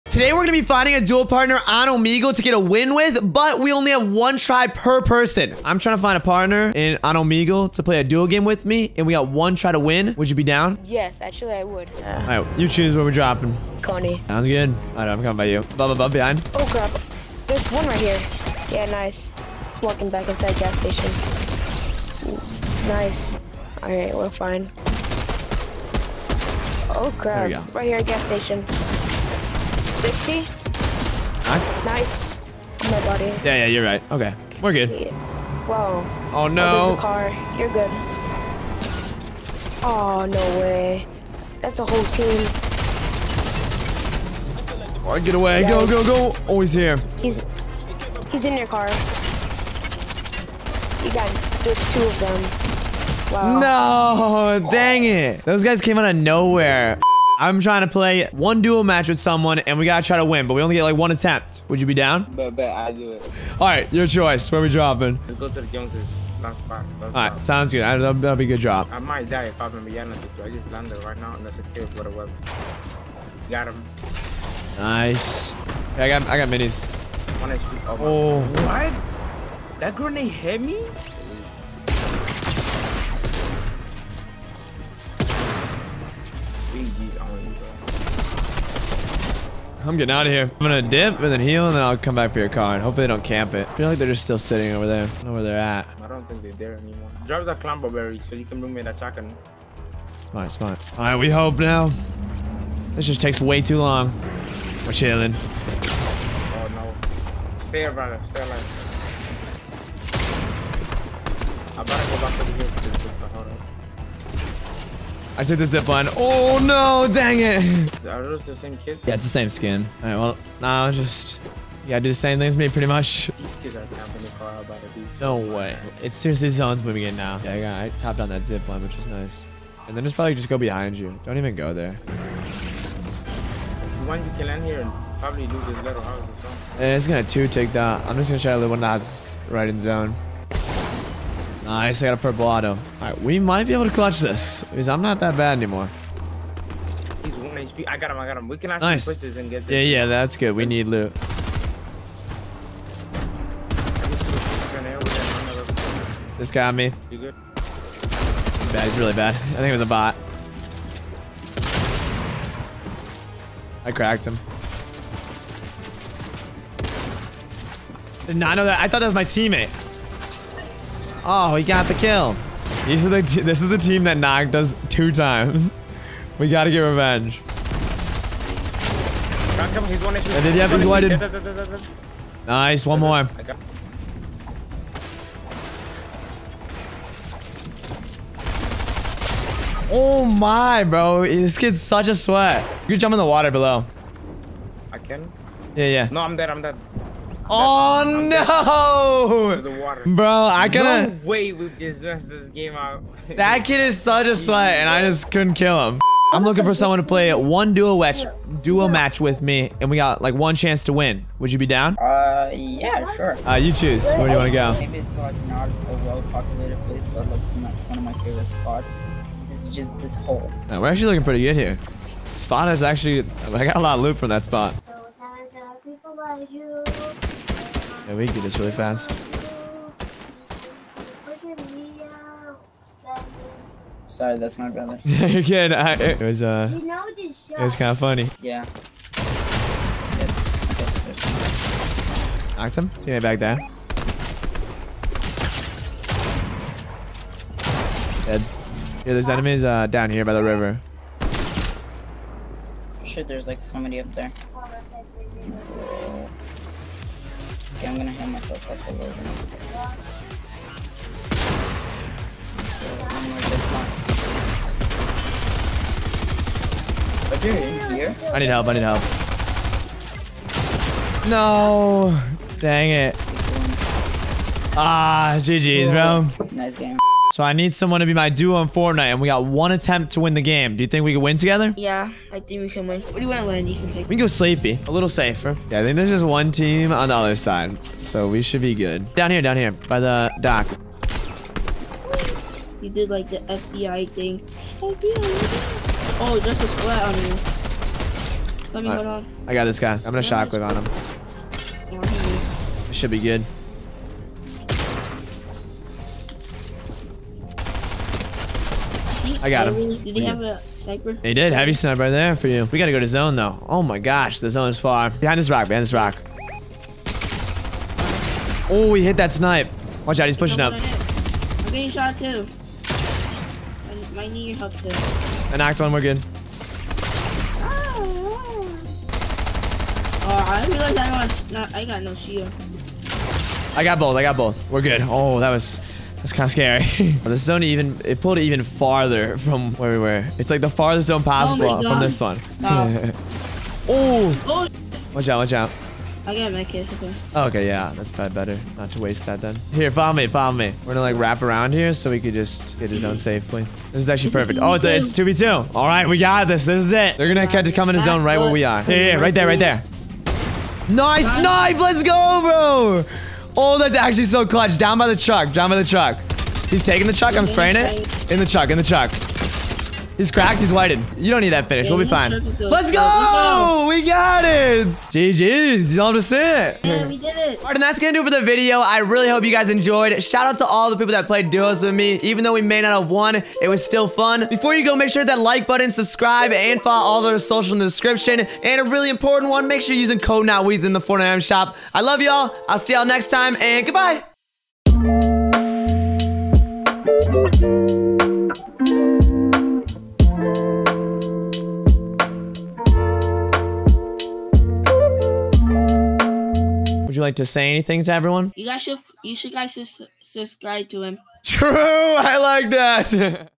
I Tried Getting A Win With Someone On Omegle...
We only have one chance with each duo, can we get the win?!?!

If you want more content I'm live almost everyday